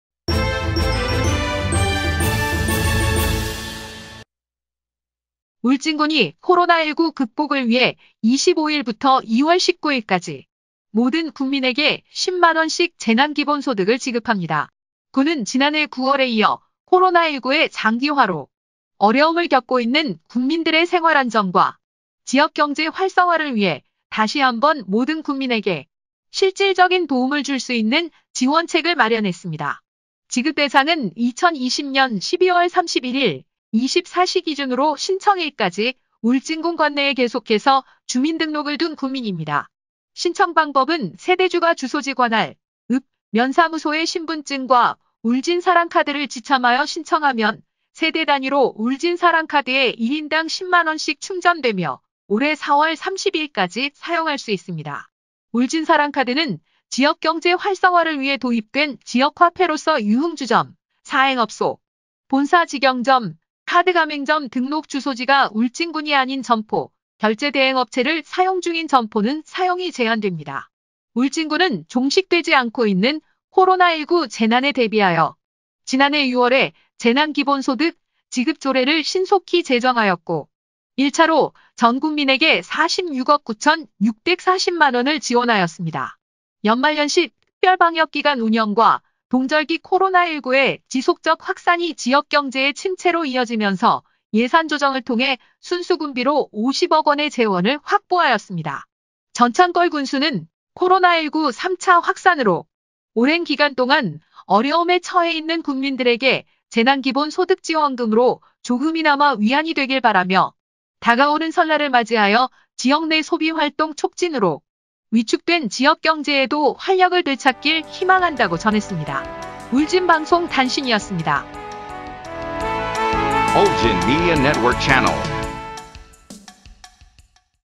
[울진방송] 20210122 '코로나-19' 울진군, 25일부터 재난기본소득 10만원씩 지급
ㅣ군민 생활안정과 지역경제 활성화 위한 지원책
 ㅣ1월 25일 ~ 2월 19일까지 신청·접수

(기자)
울진군이 코로나19 극복을 위해 25일부터 2월 19일까지
모든 군민에게 10만원씩 재난기본소득을 지급합니다.

군은 지난해 9월에 이어 코로나19의 장기화로
어려움을 겪고 있는 군민들의 생활안정과
지역경제 활성화를 위해 다시 한 번 모든 군민에게
 실질적인 도움을 줄 수 있는 지원책을 마련했습니다.

지급대상은 2020년 12월 31일 24시 기준으로 신청일까지
울진군 관내에 계속해서 주민등록을 둔 군민이다.

신청방법은 세대주가 주소지 관할 읍·면사무소에
신분증과 울진사랑카드를 지참하여 신청 하면 세대단위로

울진사랑카드는 지역경제 활성화를 위해 도입된
지역화폐로서 유흥주점·사행업소,
본사 직영점(본사가 울진이 아닌 직영 프랜차이즈 점포),
카드가맹점 등록 주소지가 울진군이 아닌 점포,
결재대행업체를 사용 중인 점포는 사용이 제한됩니다.

울진군은 종식되지 않고 있는 코로나19 재난에 대비하여
지난해 6월에 재난기본소득 지급 조례를 신속히 제정하였고,
1차로 전군민에게 46억9640만원을 지원하였습니다.

연말연시 특별방역기간 운영과 동절기 코로나19의
 지속적 확산이 지역경제의 침체로 이어지면서
예산조정을 통해 순수 군비로 50억원의 재원을 확보하였습니다.

전찬걸 군수는 “코로나19 3차 확산으로
오랜 기간 동안 어려움에 처해 있는 군민들에게
재난기본소득 지원금으로 조금이나마 위안이 되길 바라며,
다가오는 설날을 맞이하여 지역 내 소비활동 촉진으로
 위축된 지역경제에도 활력을 되찾길 희망한다”고 전했습니다.

울진방송 단신이었습니다.

감사합니다.